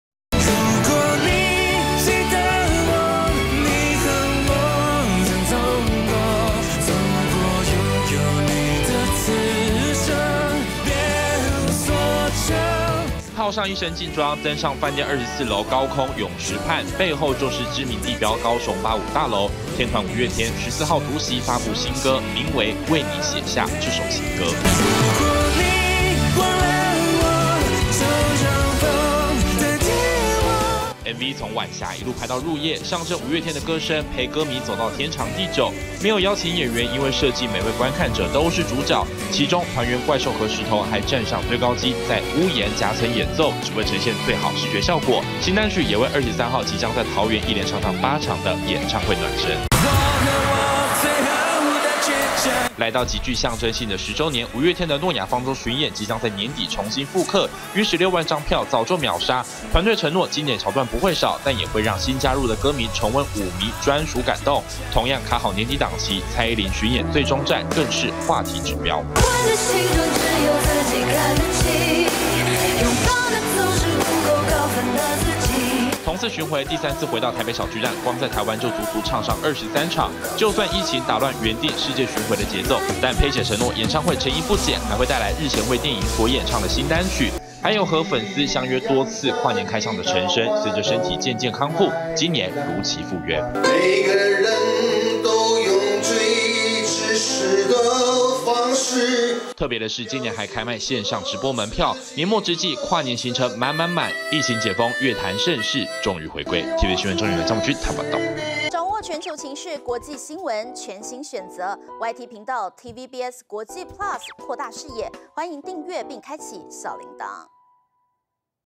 【星聞爆爆】五月天送給「五迷們」的情歌！新MV「登24樓泳池」暖身個唱
疫情步步解封，也讓今年各地'跨年行程'顯得更加精采，包含蔡依林、陳昇、五月天等歌手，都要接連'舉辦'跨年演唱會；尤其五月天'更在14號晚間上架最新單曲，紀錄和歌迷共同創造的美好回憶，MV是到高雄飯店取景，不少人直呼'真的美翻。

2013年TVBS歡樂台第二部原創概念劇